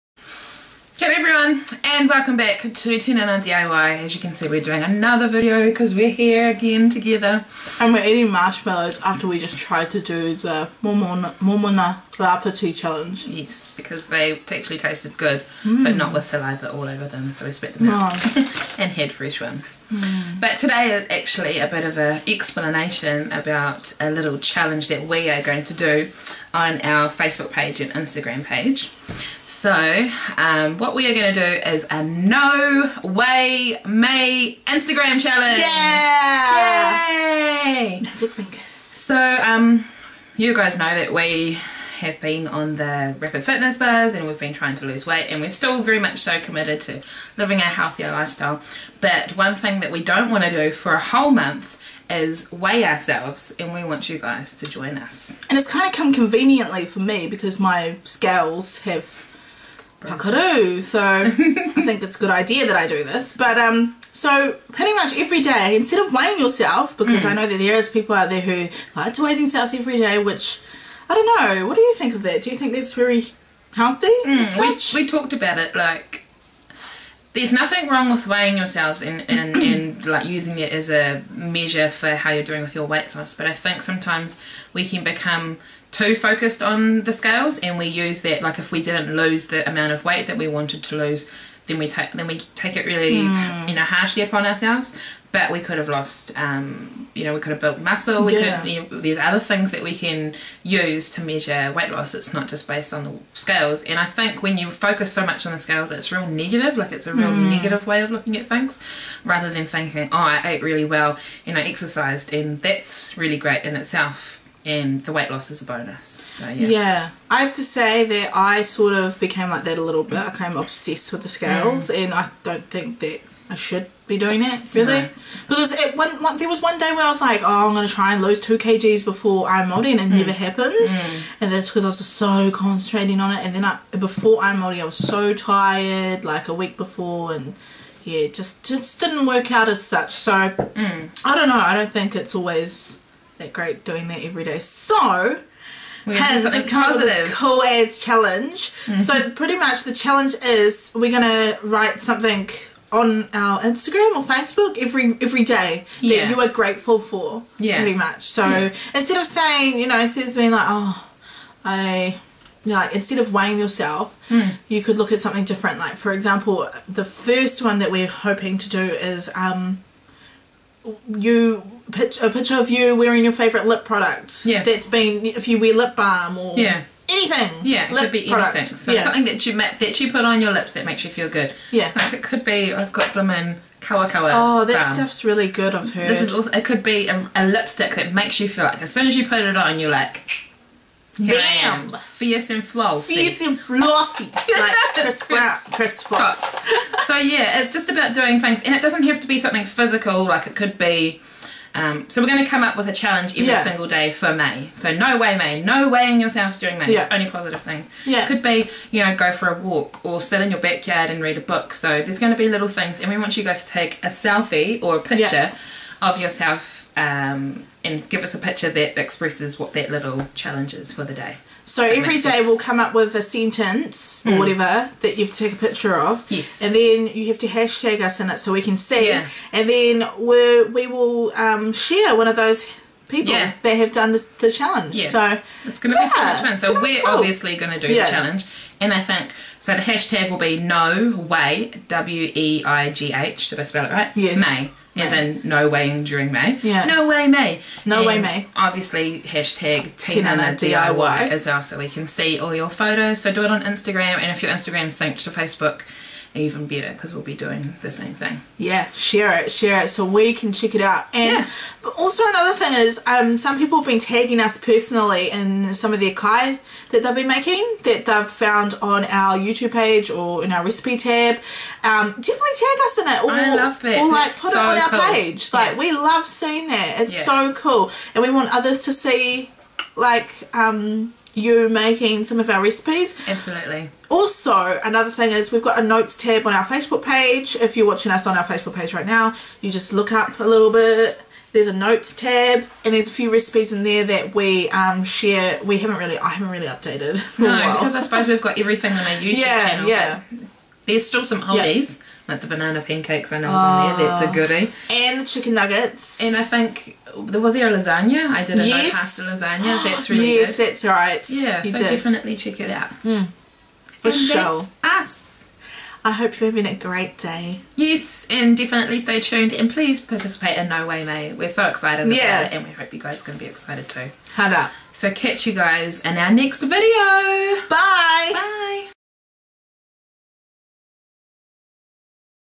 NO WEIGH MAY CHALLENGE!! | Instagram Challenge!
Kia Ora

This is an awesome new challenge we have decided to do for the whole month of May 2014.

We wanted to do something where we encouraged people to find something they love about themselves each and every day.

We want you to share with us as we believe everyone is beautiful no matter what!

So watch out for our instagram challenge - NO WEIGH MAY! We will be starting on the 1st May 2014. Check out more details below to join us in the challenge.

Much Aroha,

Hinz & Leah x

Have you checked out our other social media sites?